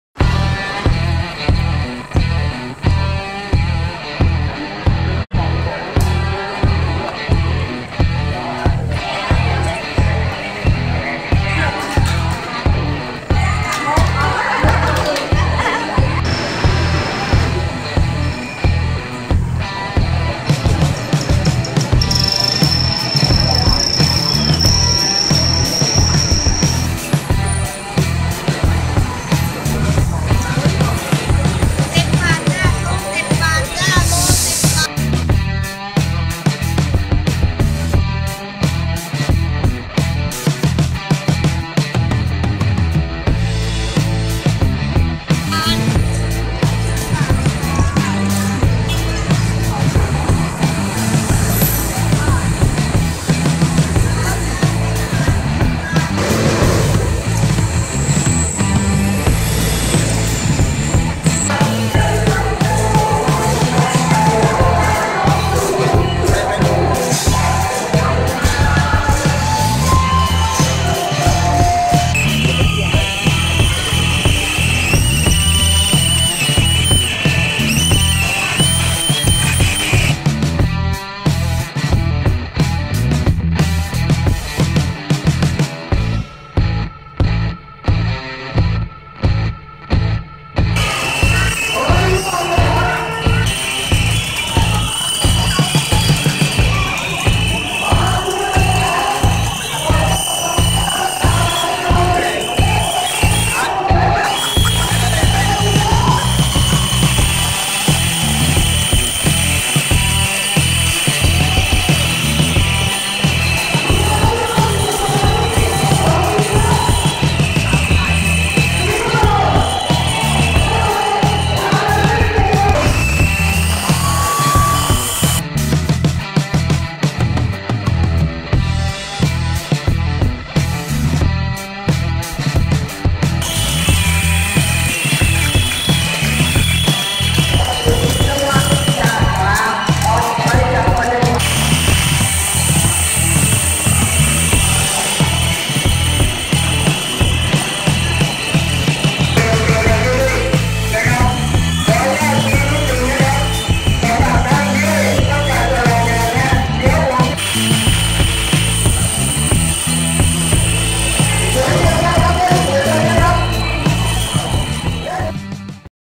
Amnesty-bill-protesters gathering at Asoke
Today again protesters against the government bill gathered at Asoke at noon. They later walked up Sukhumvit to join the march of other protesters.